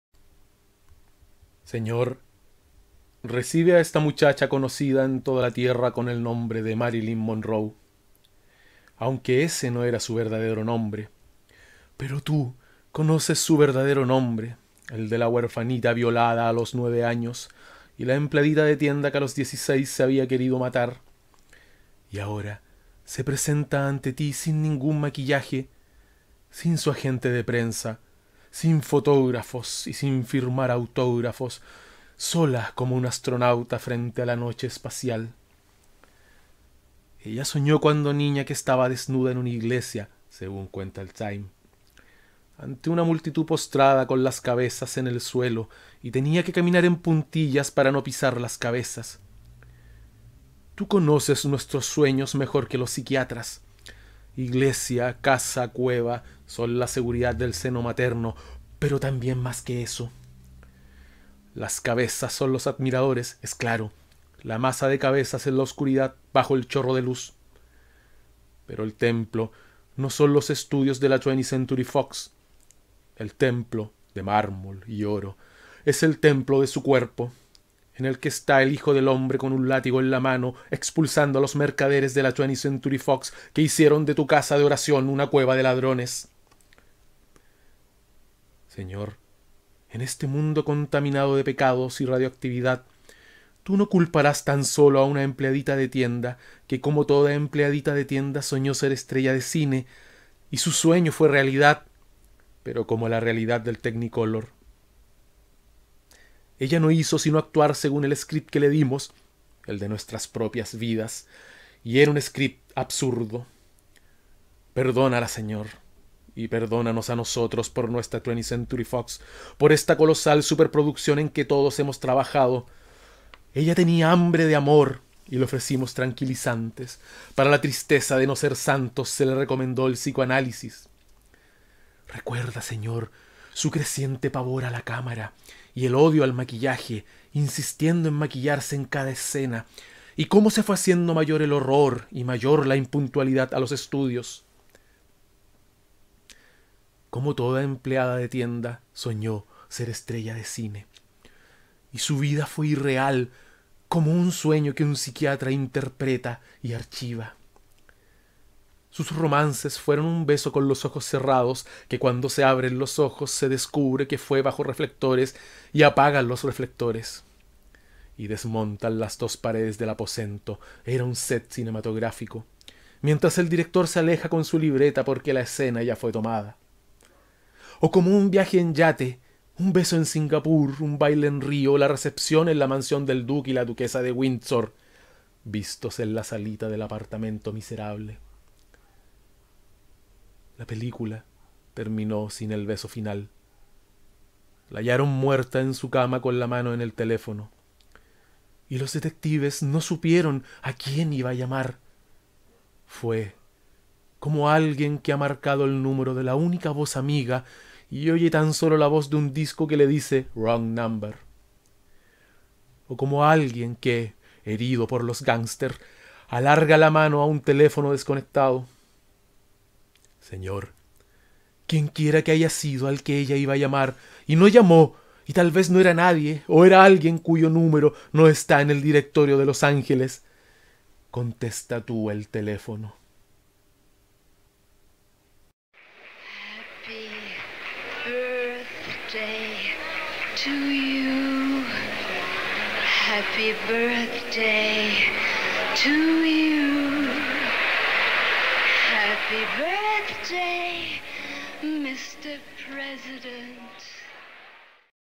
"ORACIÓN POR MARILYN MONROE" - de Ernesto Cardenal
En 1965, tres años después de la muerte de Marilyn Monroe, el poeta nicaragüense Ernesto Cardenal publicó este tributo, donde de manera descarnada, nos presenta la verdadera cara de esa fantasía que solemos llamar "estrella".

Lectura por Pablo Octavio Rossel.